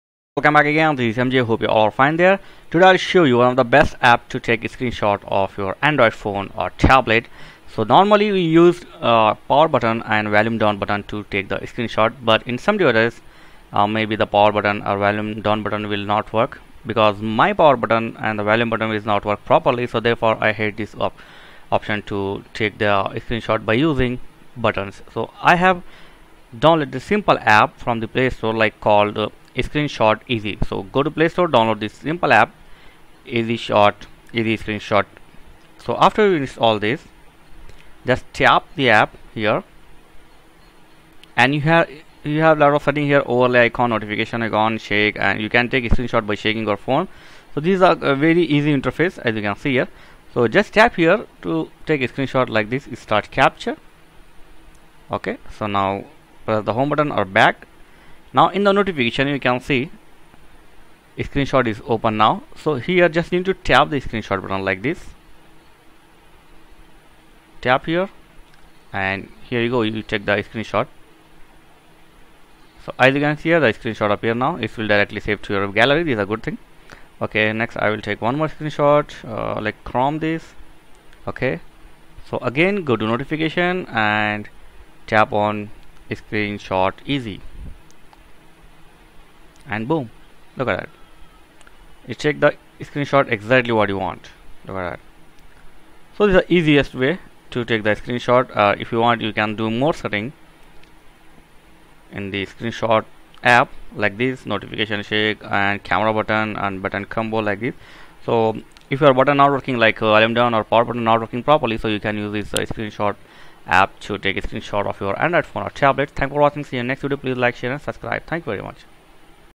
Best App to Take Screenshot for Android Phone & Tablet
Take phone screenshot without pressing buttons